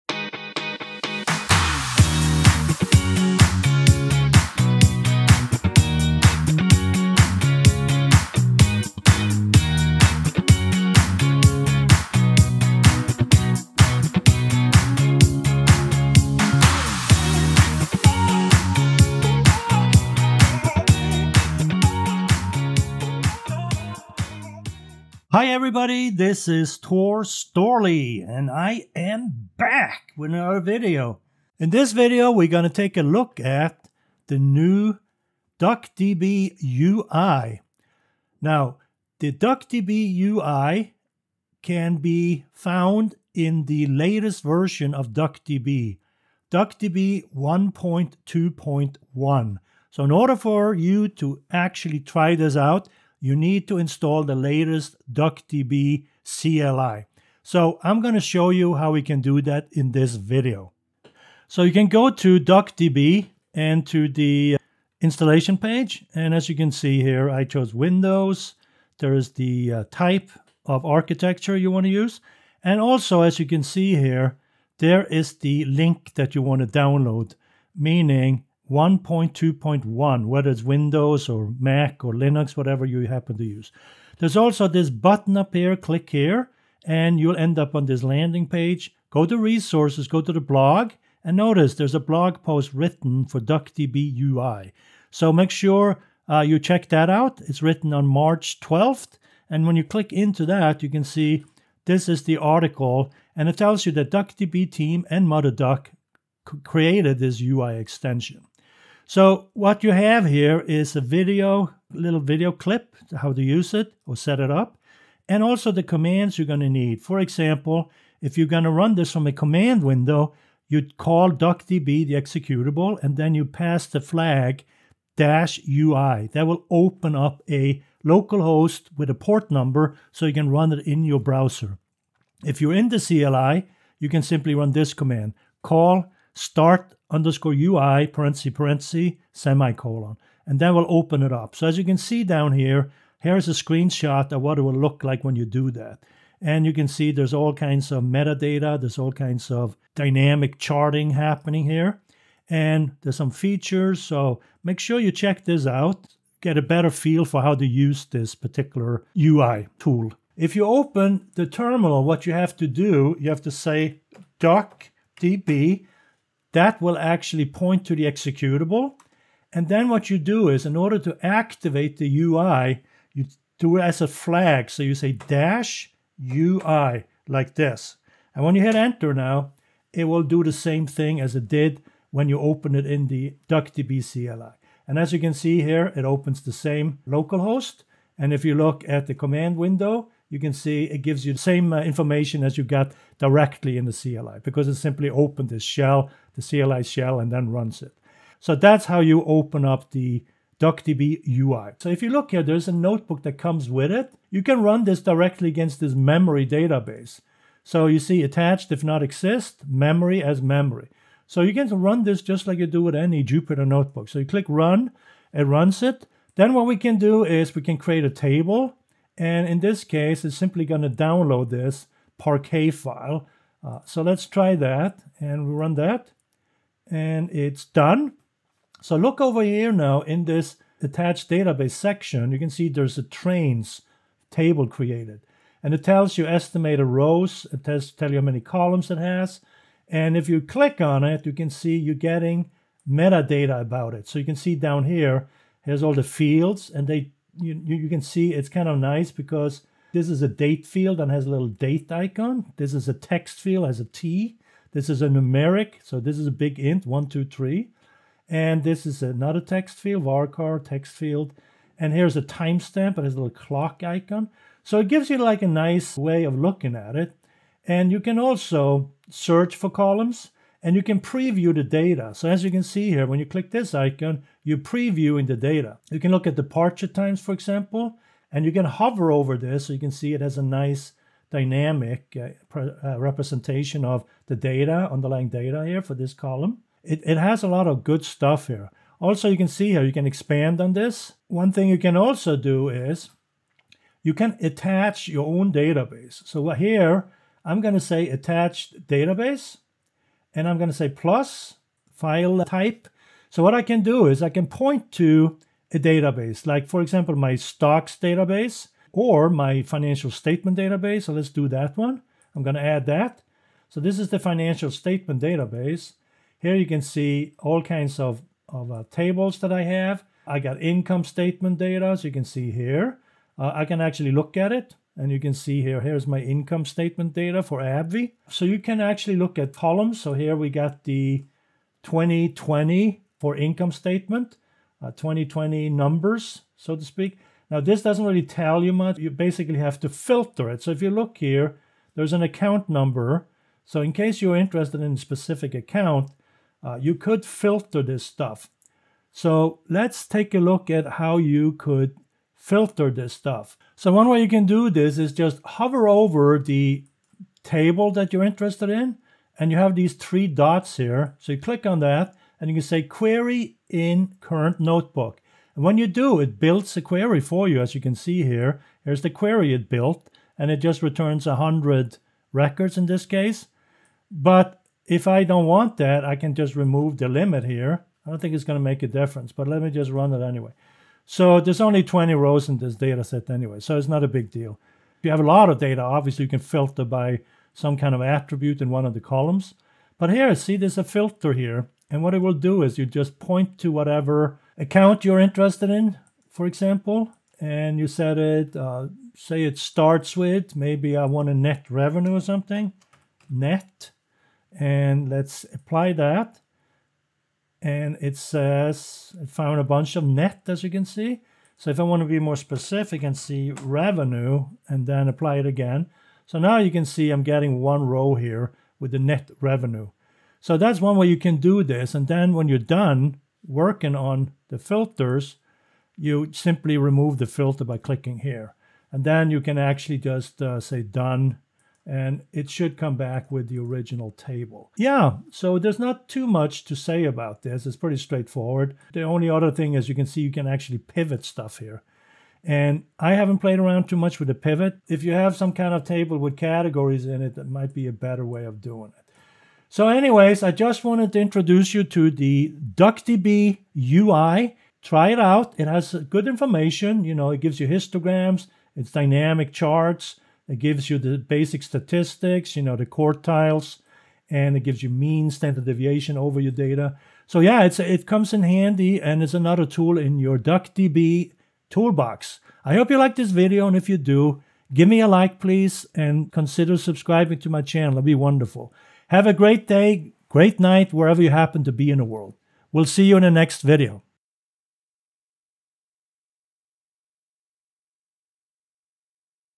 Introducing DuckDB UI
In this video, we will introduce the new DuckDB UI Extension that can be launched from the DuckDB CLI.